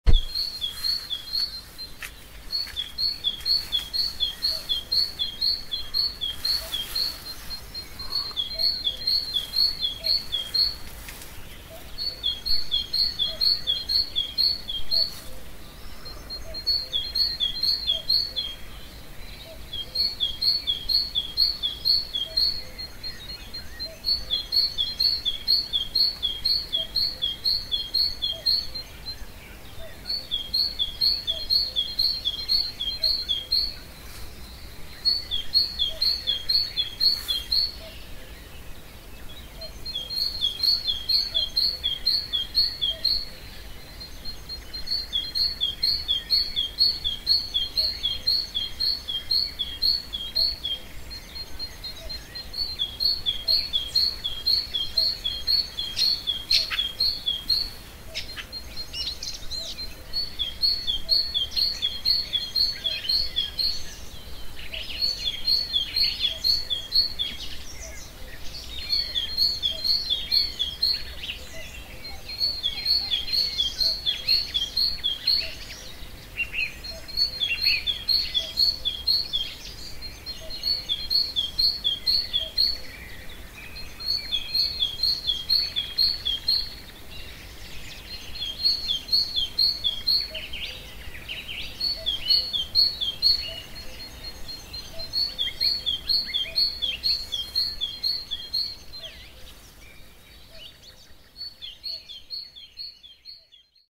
Привет от лесных подснежников поздней весной. Greetings from forest snowdrops in late spring.
Подснежник, цветок которого очень популярен среди населения ввиду своей красоты и оригинальности,  – лесной цветок и в диком виде был обнаружен в лесистых территориях Турции и Европы. Эти изящные цветы можно часто увидеть выглядывающими из снежного покрова для цветения поздней зимой или ранней весной. И особенно, приятно их встретить в лесу в этом году поздней весной. На видео лесной цветок подснежника, пение лесных птиц.